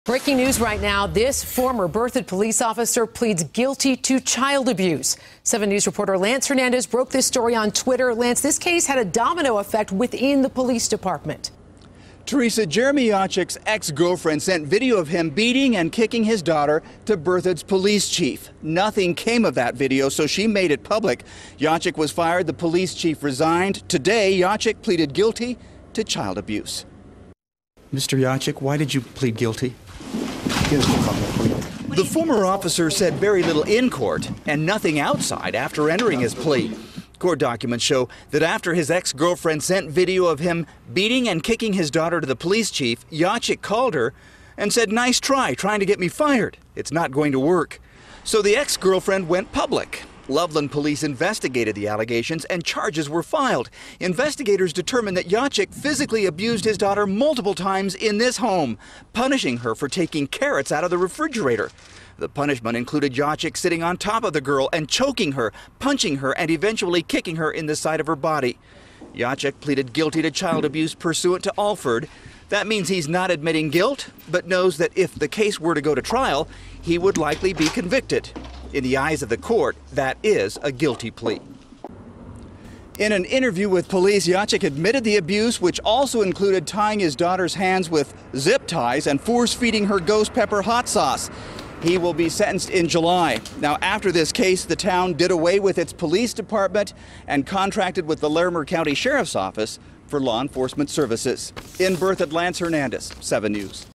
Fired Berthoud cop pleads guilty to child abuse
Fired Berthoud police officer Jeremy Yachik pleaded guilty Friday to one count of child abuse.